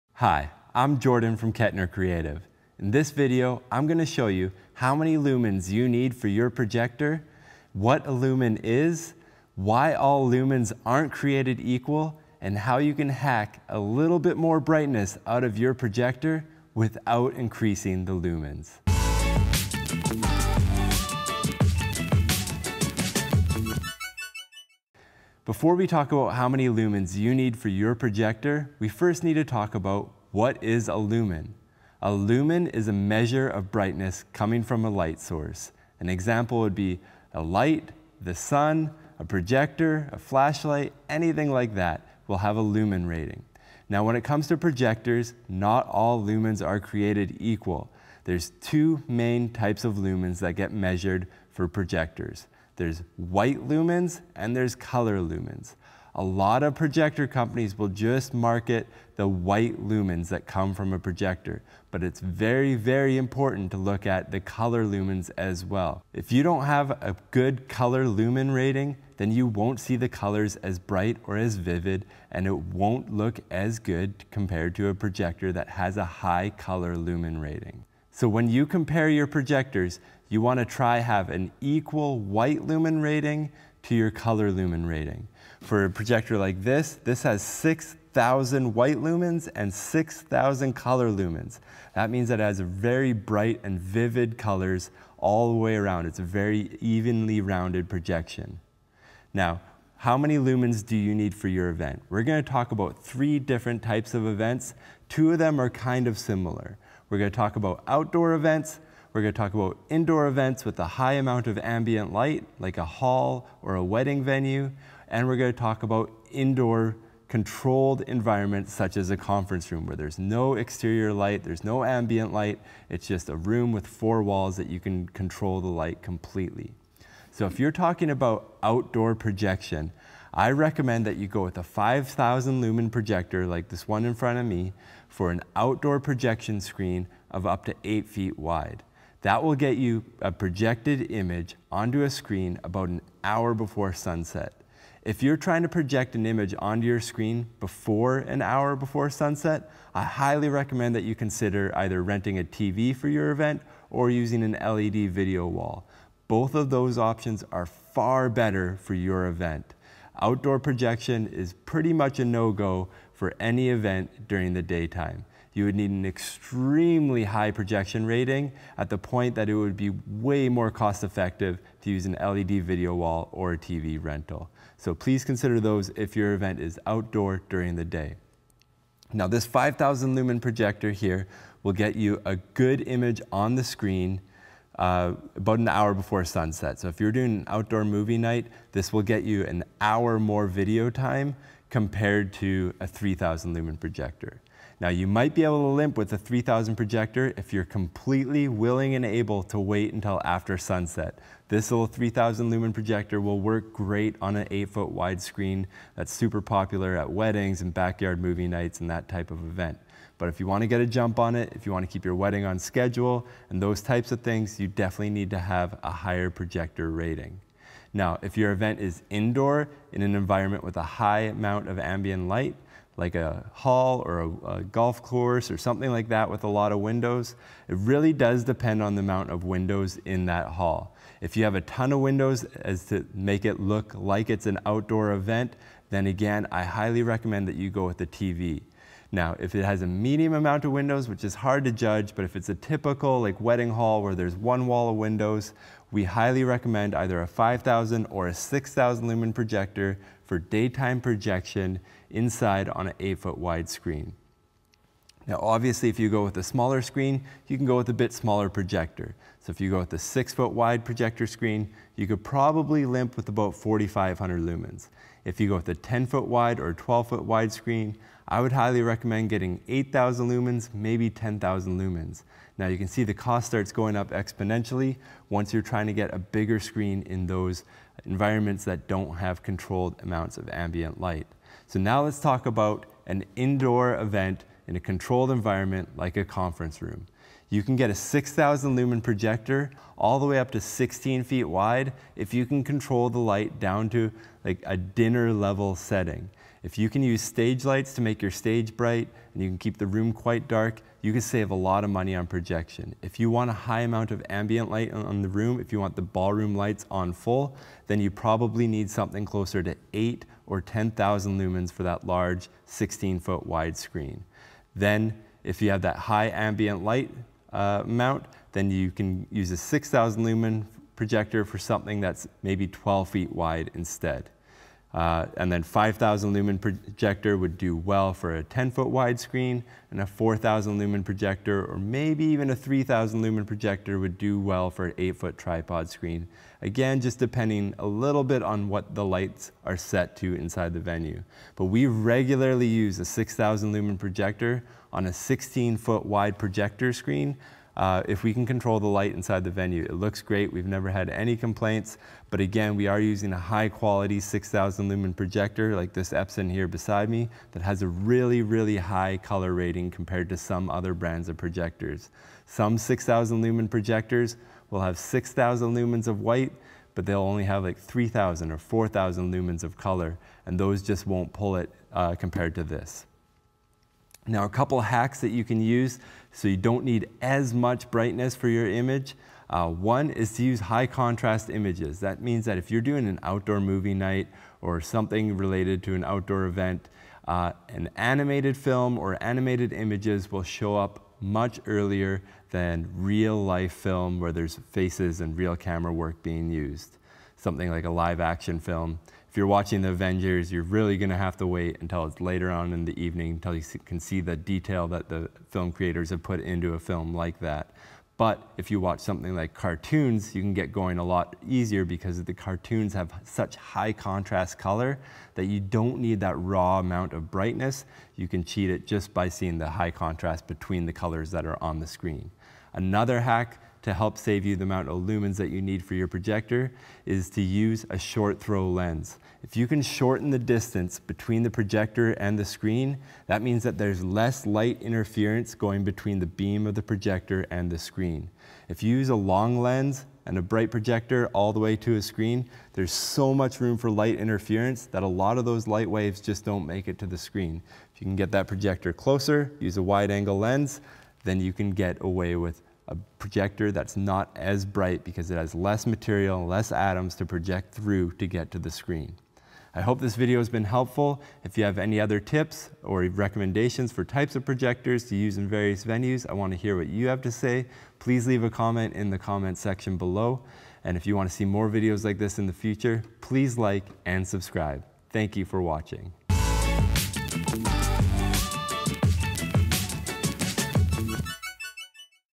How Many Lumens Does My Projector Need?
In this video, we discuss exactly how many lumens your projector will need in various types of events. We also discuss what a lumen is and why colour lumens are important. At the end of the video, we give you two tips for getting brighter images without needing extra lumens.

CHAPTERS:
0:00 - Introduction
0:30 - What Is A Lumen?
0:53 - White Lumens VS Color Lumens
2:07 - Outdoor Events
3:38 - Indoor Events With Windows
4:42 - Indoor Events With No Windows
6:20 - High Contrast Images
7:02 - Short Throw Lens
7:40 - Final Thoughts